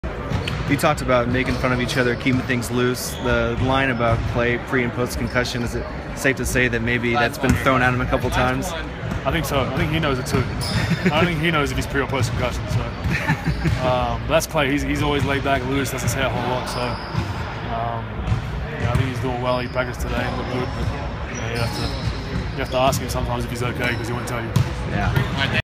Andrew Bogut on Klay Thompson after a concussion
Before the NBA Finals, Golden State Warriors center Andrew Bogut talks about Klay Thompson and how much he was affected by a concussion suffered against the Houston Rockets.